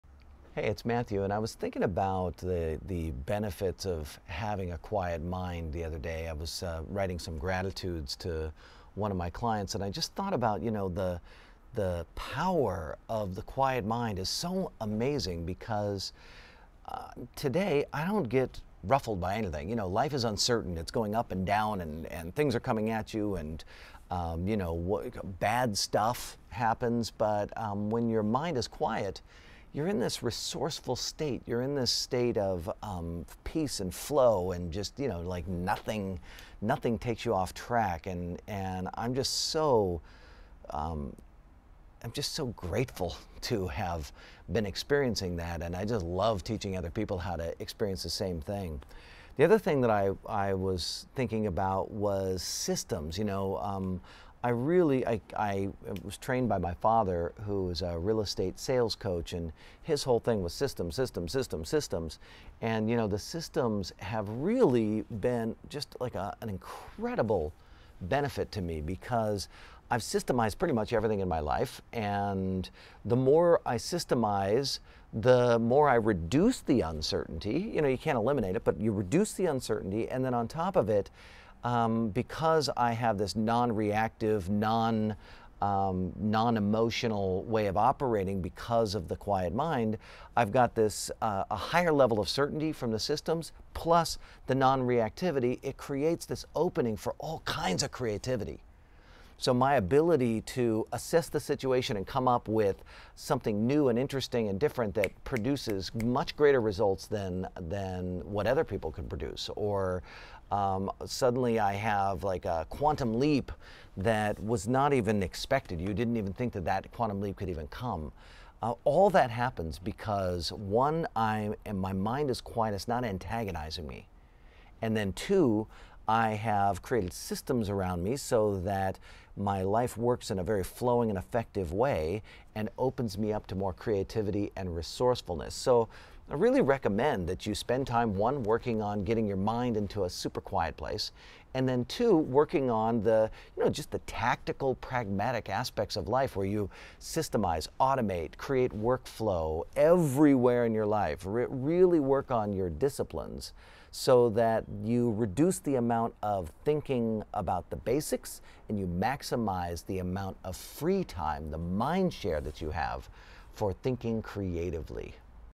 Quiet Mind + Structure = Freedom
When your mind goes quiet, you aren't antagonized by the mind chatter. If you add structure and discipline to that equation, suddenly you reduce uncertainty and now your creativity comes through. This is a recipe for profound freedom.